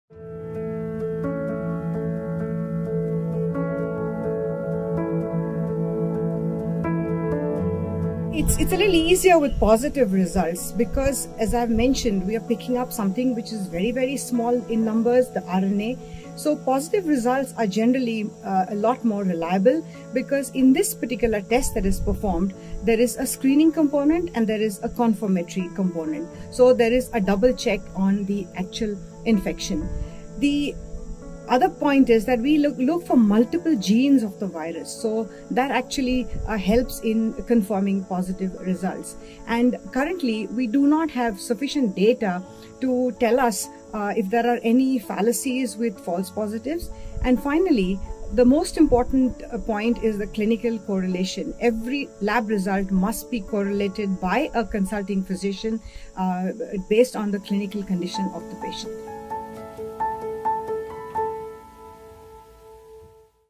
Doctor's Speak: How Reliable is a Positive COVID-19 Report?
How reliable is a positive COVID19 report? Answerd by Dr. Rohini Kelkar, Clinical Microbiologist and former HOD at Tata Memorial center.